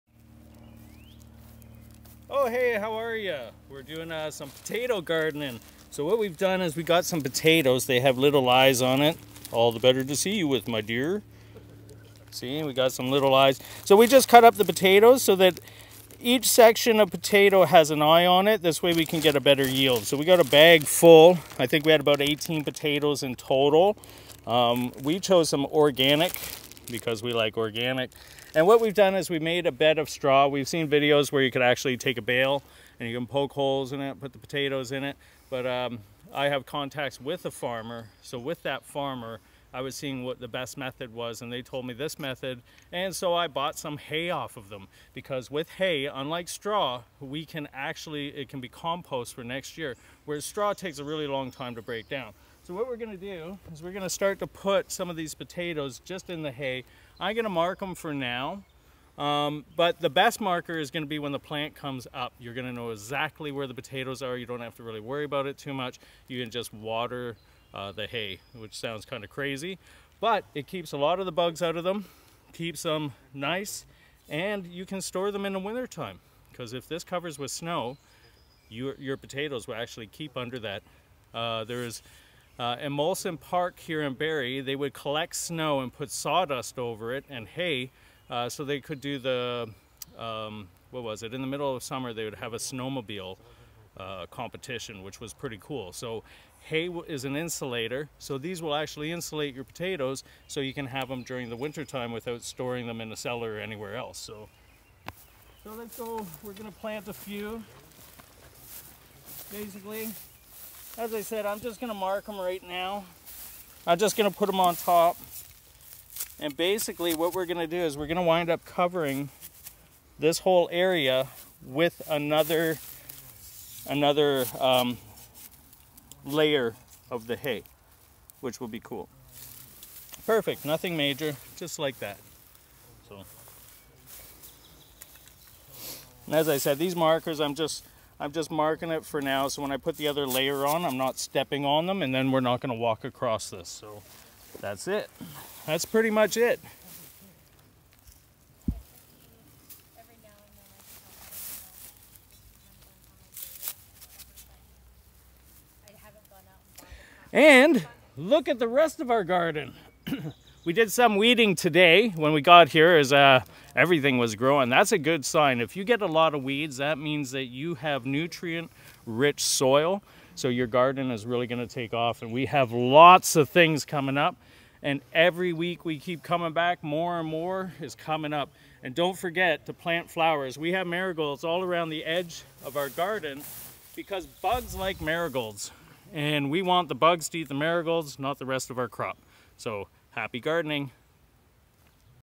The Community Garden Saga - You did what to the potatoes?!?
The community garden is coming along. We added a new crop. Investigated the crop circles and new farming methods. We also welcomed badly needed help. Grow a garden - your stomach will thank you. it is in our humble opinion better to grow a garden than a lawn. Exercise your rights, get exercise, meet new people, learn new skills, by volunteering at a community garden. This our week 6 update.